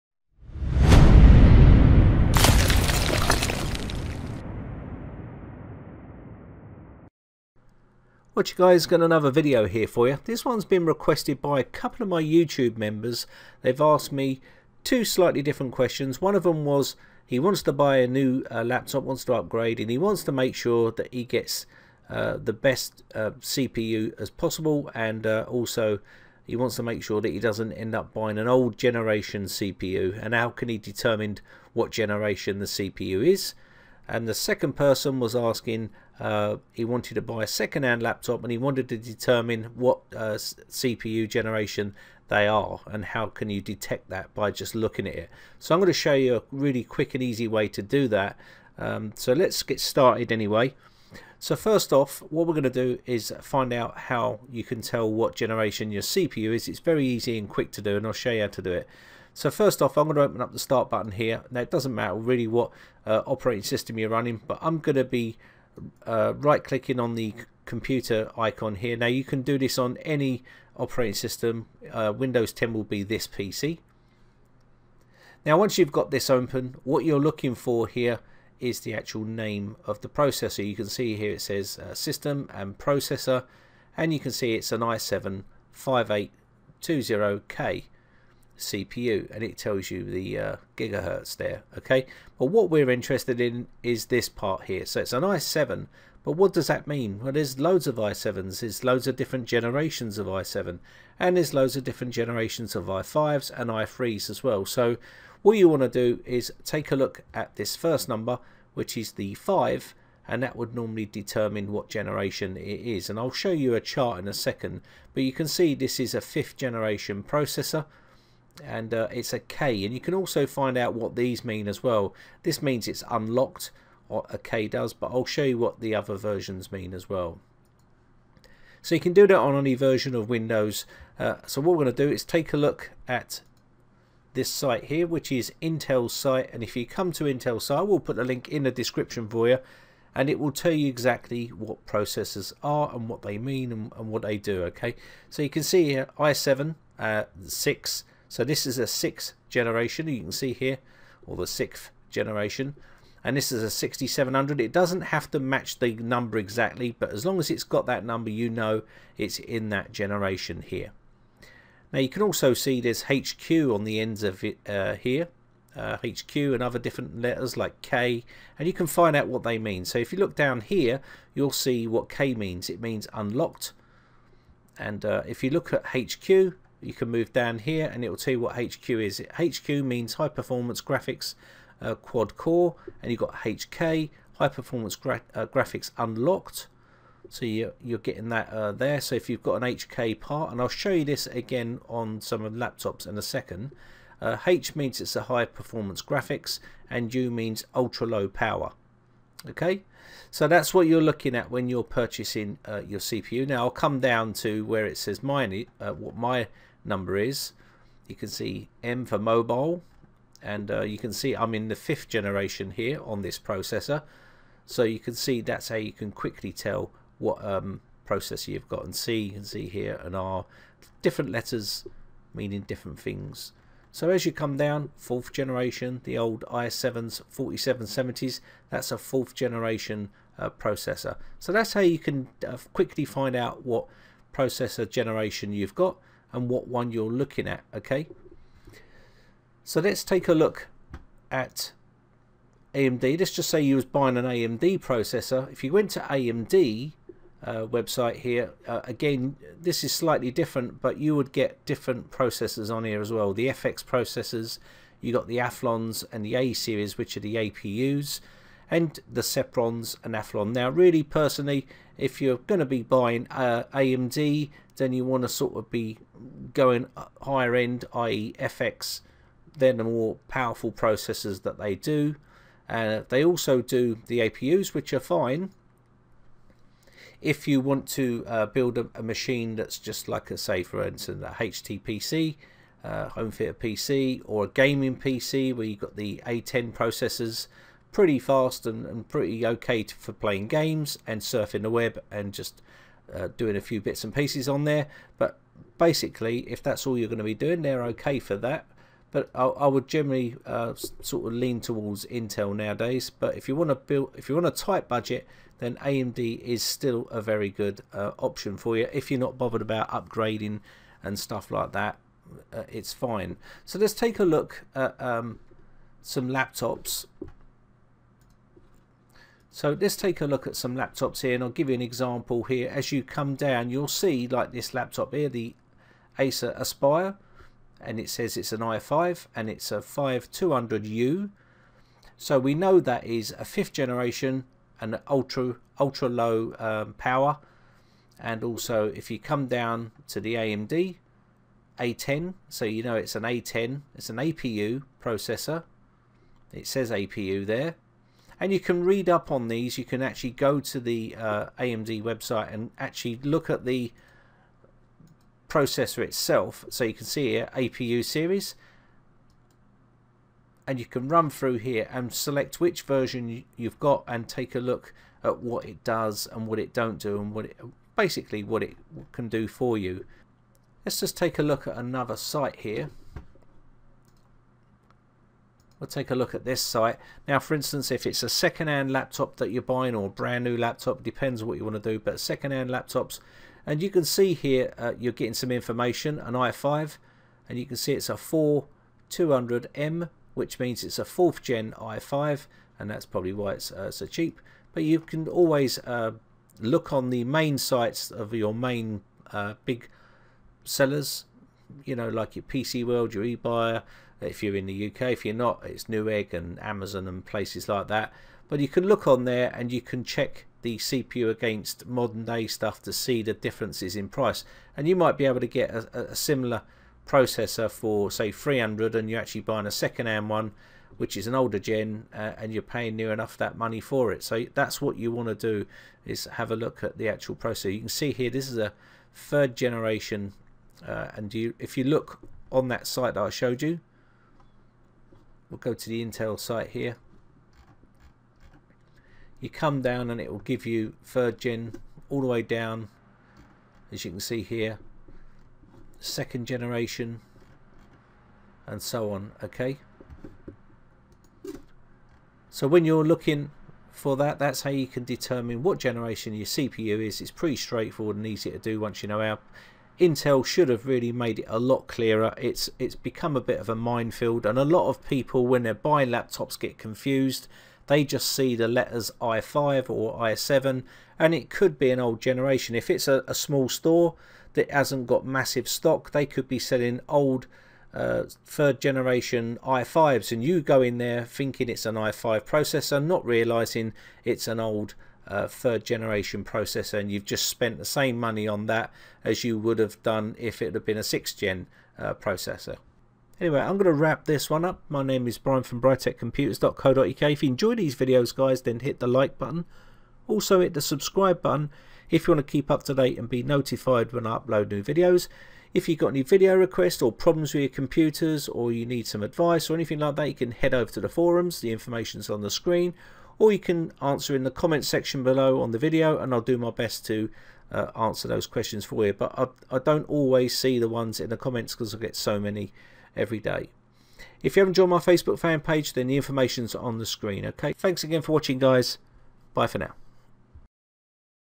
What Generation is My CPU Processor
If you looking to buy and new or second hand computer or laptop, you might want to know what CPU is in that machine, there is so many CPU's on the market nowadays i3, i5, i7, FX, APU, Sempron,  athlon, A4, A5, A6, A8, A10 and it can be very confusing to some users or buyers.

I will show you how to find what generation Intel Processor or AMD processor you have or what you want to buy. Here how to find your Intel Processor generation.

For Windows 7 Click Start button and Right-click My Computer and select Properties, now you will see a windows called "view basic information about your computer" its a basic summary of the hardware and operating system. The information  what we are interested in is which processor you’re running.

If you are running Windows 8 or 10. Right-click This PC, select Properties.

Now you can go to Intel or AMD website and look for your processor code.

If you need any help with computer problems, please join my forum